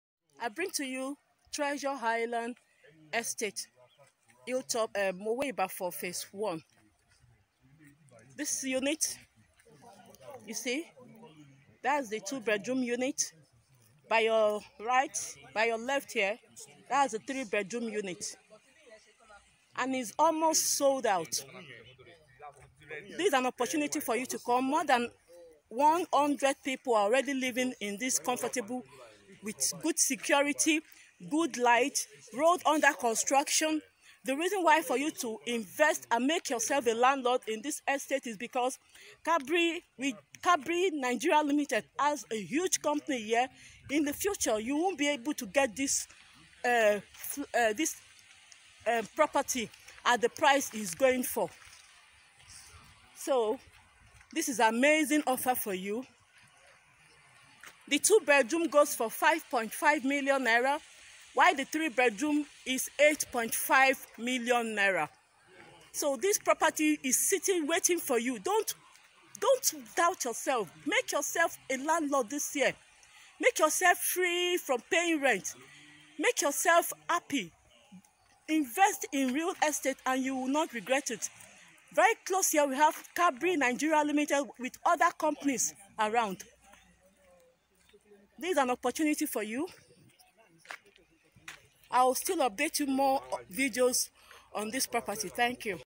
Treasure Island Estate is the first Nigerian Afforfable Estate on mainland MOWE/OFADA Ogun state,
Treasure Island Estate is planned to be the first Nigerian Afforfable and Aesthetics Estate on the mainland MOWE OFADA Ogun state,

Which part of the Treasure Island Estate, Mowe/Ofada are you interested in? *

Plot(s) of land

2 bedroom bungalow (Semi detached)

3 bedroom bungalow (Fully detached)

3 bedroom duplex

4 bedroom duplex

Commercial Plot

Others

LEGAL DOCUMENTS

• Deed of Contract
• Deed of Assignment (Registered)
• Certificate of occupancy
• Survey

INFRASTRUCTURE

• Earth Road
• Electricity
• Drainage
• Water
• CCTV
• Perimeter Fencing
• Basket Ball court
• Children Playing Ground

NEIGHBORHOOD

• International Brewery
• Nestlé Industries
• Queens Park
• Havillah Estate
• Redemption Camp

ESTATES WITHIN

• Treasure Homes